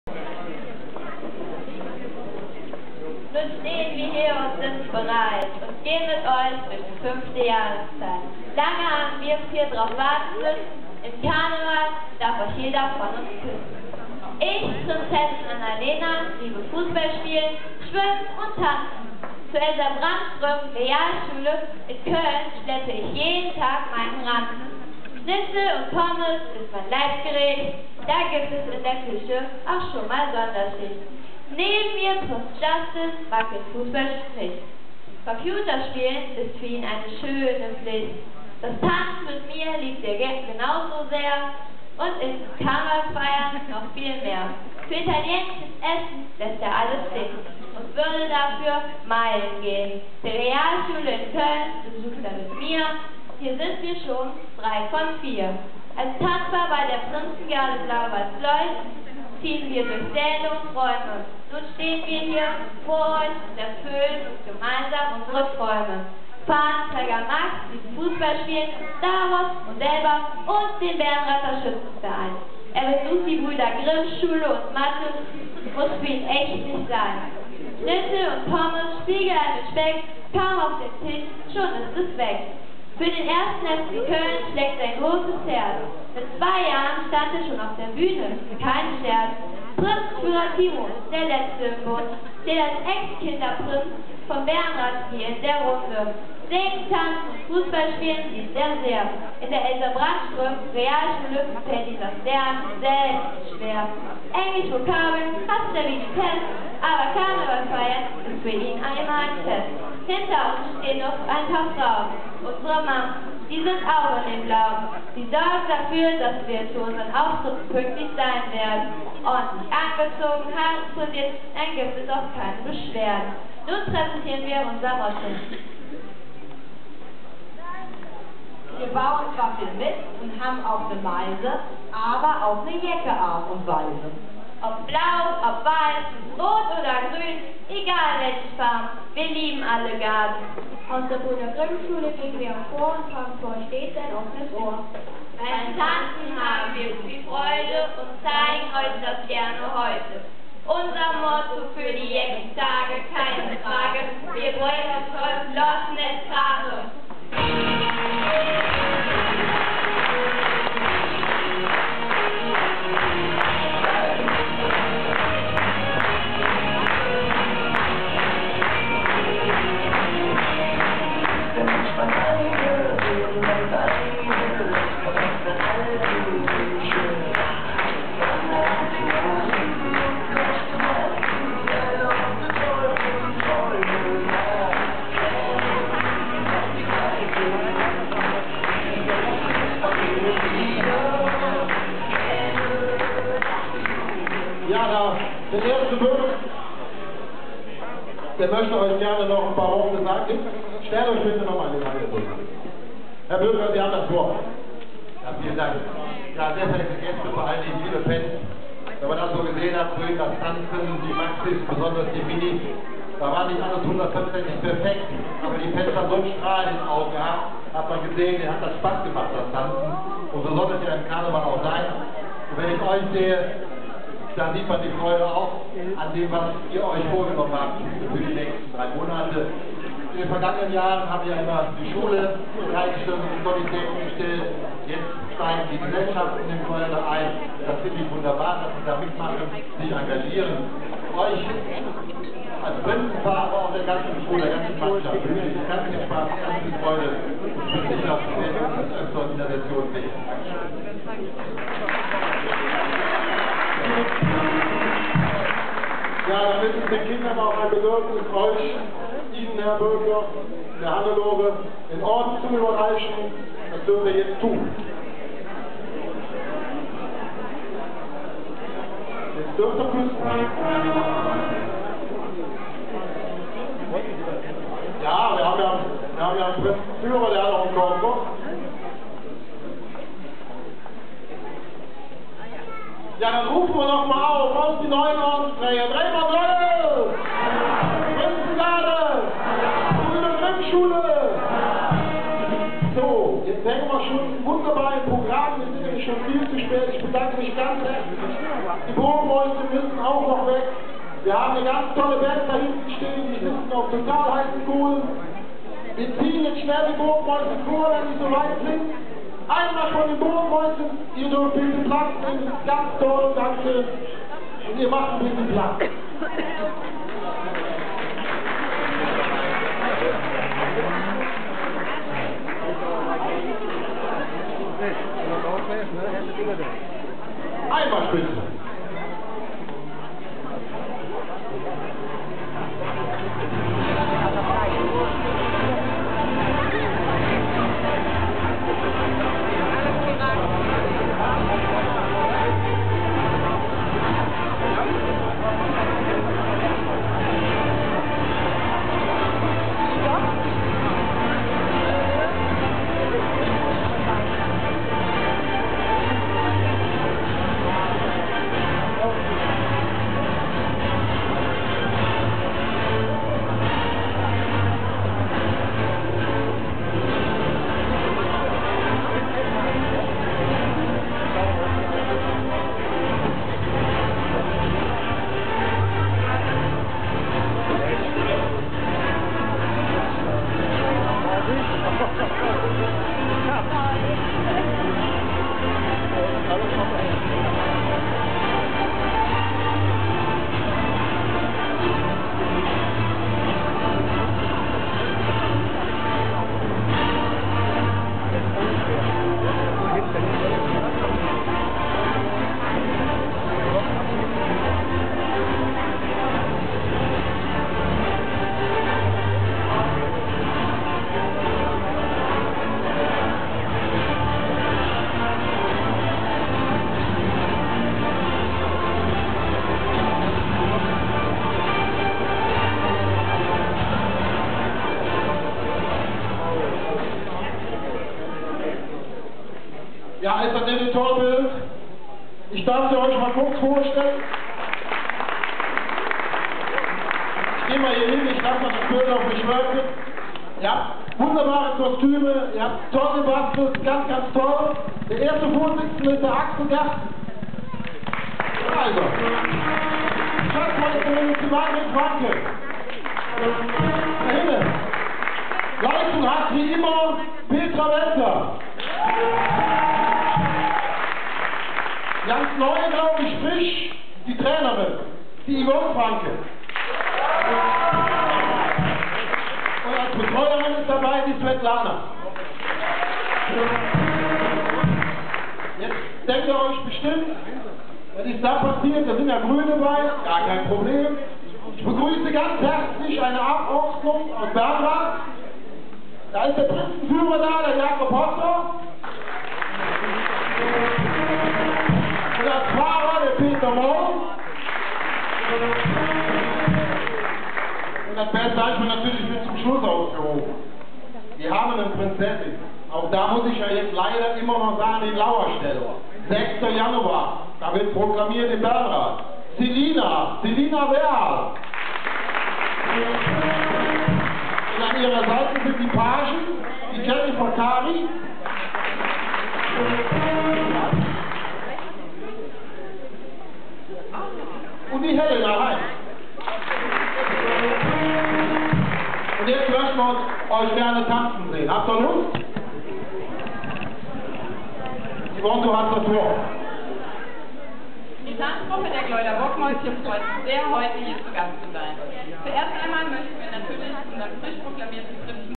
Kinderproklamation Brüder Grimm Schule 2012-13
Proklamationsrede vom neuen Kinderprinzenpaar und die ersten Ordensverleihungen.
Aufwartung der Tanzgruppe aus Hürth Berrenrath der Burgmäusschen als Piraten aufmarsch.
Dann zeigten Sie Ihr können für das neuen Prinzenpaar und Gäste  die zur Proklamation gekommen waren.